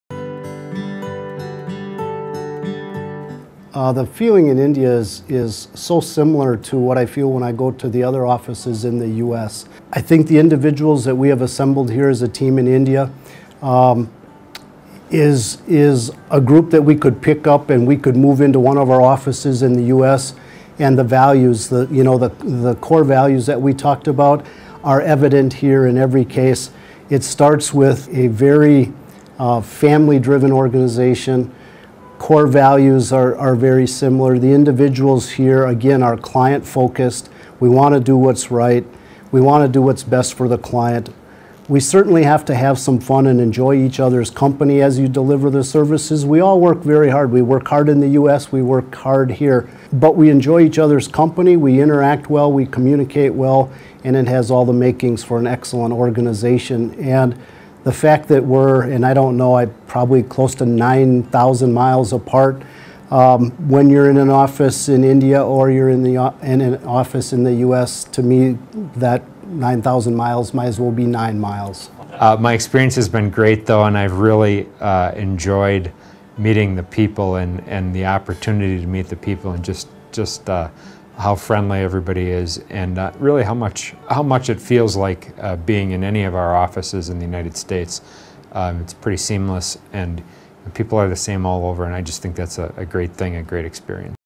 Leaders Speak 3: Rick Dreher & Jeff Kowieski, Wipfli LLP
In this Webisode of 'Leaders Speak', watch Rick Dreher, CPA, Managing Partner, Wipfli LLP and Jeff Kowieski, CPA, Partner, COO, Chief Compliance Officer, Wipfli LLP talking about their experience in India. This is the 3rd part of the Leaders Speak Series.